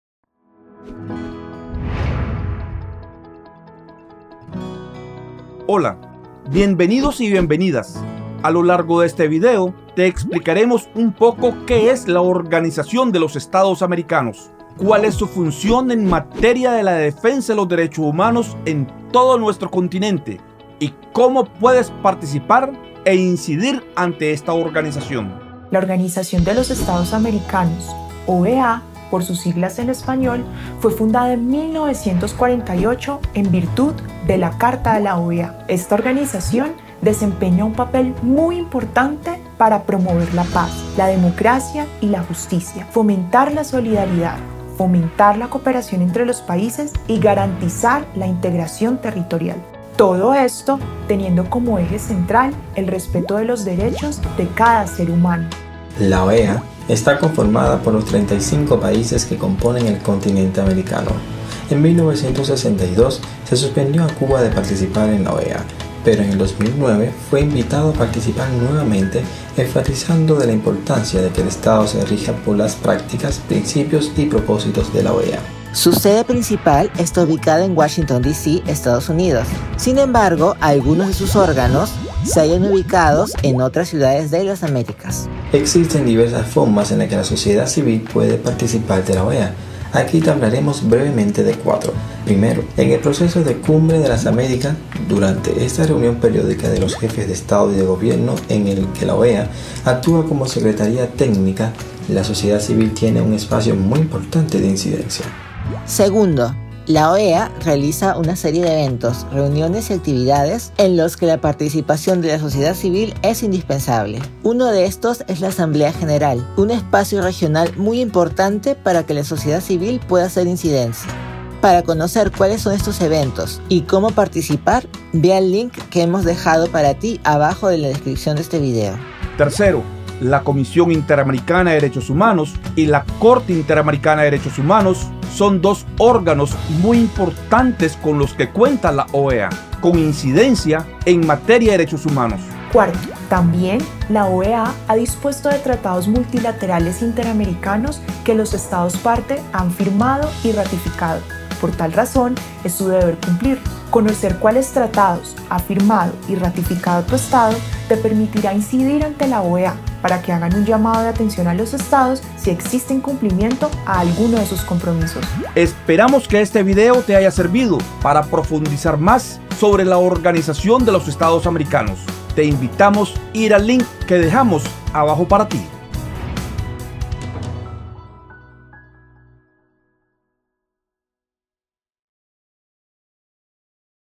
Qué es la Organización de los Estados Americanos (OEA)
A lo largo de este video te explicamos qué es la Organización de los Estados
Americanos (OEA) y cuál es su función en materia de la defensa de los derechos humanos en todo nuestro continente.

Race and Equality - Agosto 2020